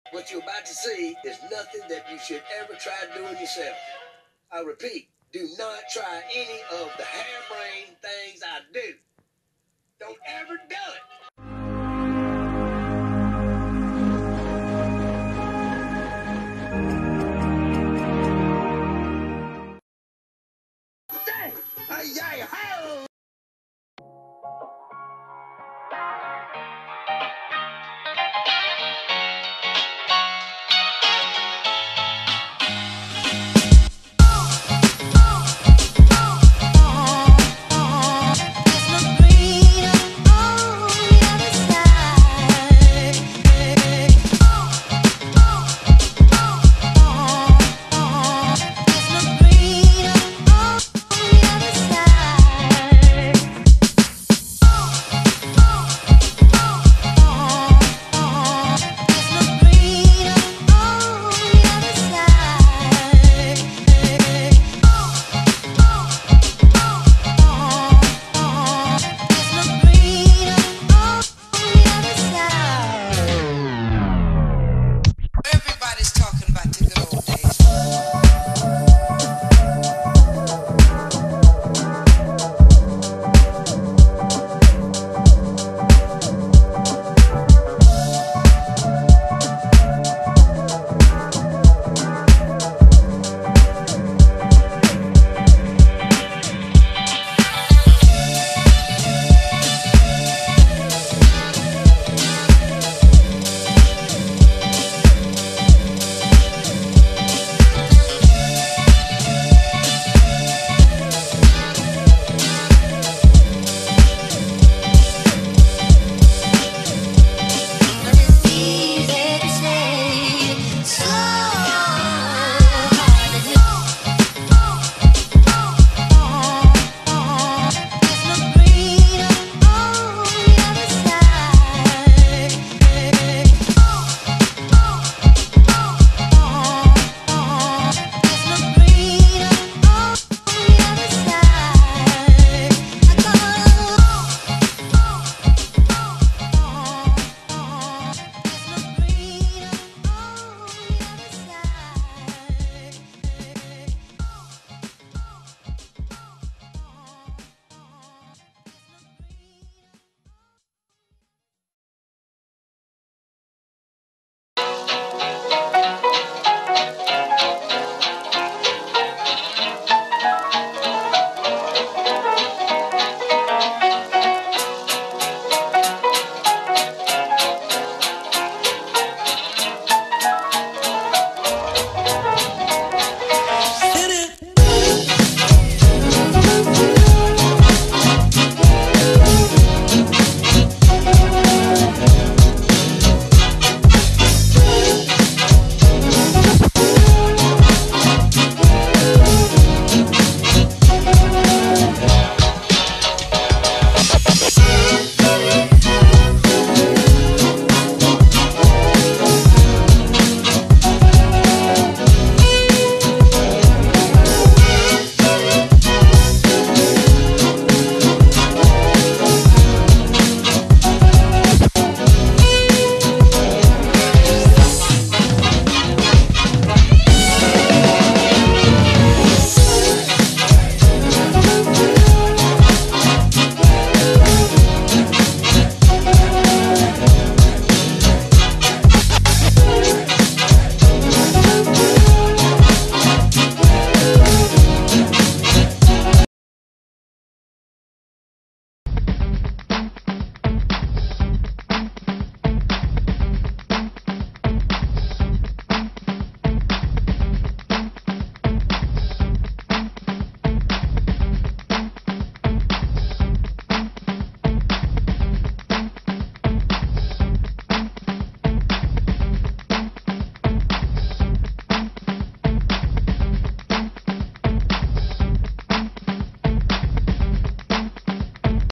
Source1 Frame Baduga build never before seen footy and a few shout outs
Straight from the Vault!! some long lost fpv footy never seen on youtube, Enjoy! Flying the Source1 Frame Baduga build 5 inch Quad Copter.